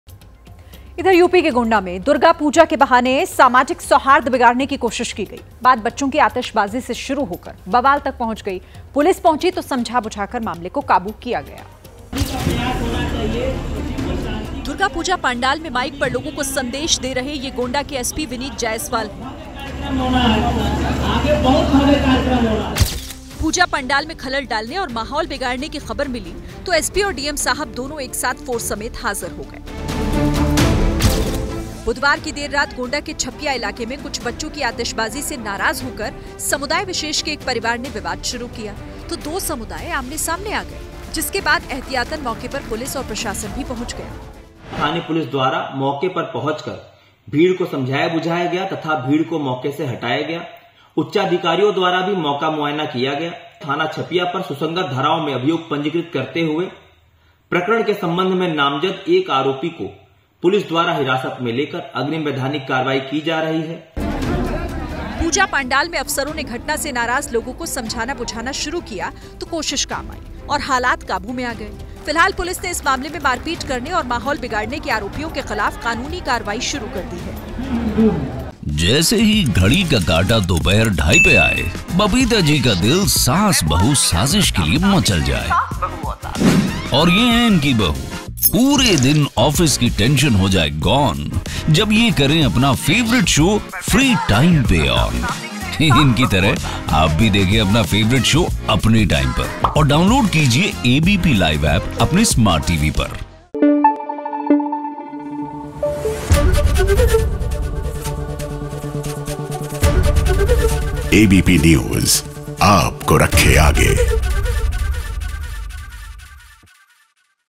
Durga Pooja: गोंडा में दुर्गा पूजा के दौरान माहौल बिगाड़ने की कोशिश | ABP News
ABP News TV | यूपी के गोंडा में दुर्गा पूजा के बहाने सामाजिक सौहार्द बिगाड़ने की कोशिश की गई। बात बच्चों की आतिशबाजी से शुरू होकर बवाल तक पहुंच गई। पुलिस पहुंची तो समझा-बुझाकर मामले को काबू किया गया ।